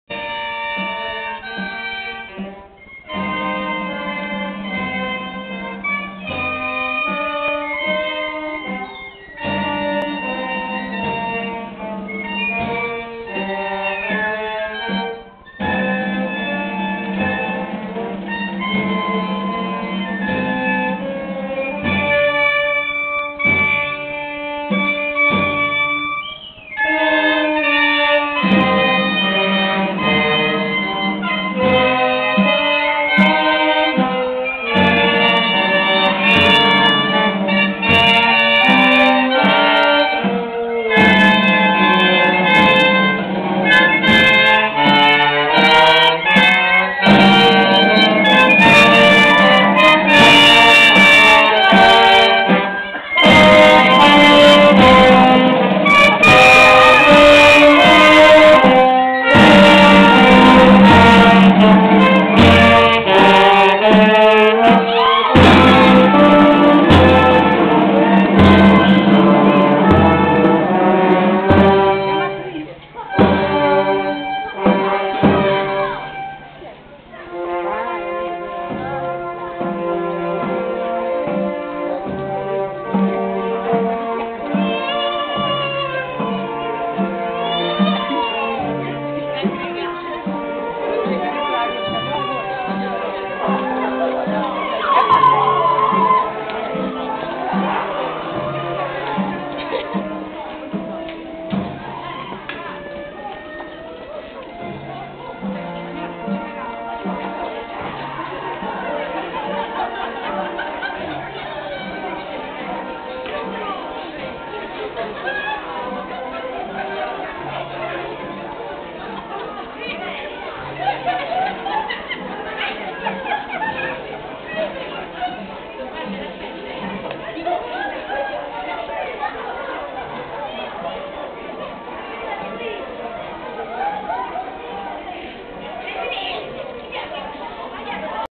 Carnevale Monteroduni '08
Altra divertente ripresa della processione!!!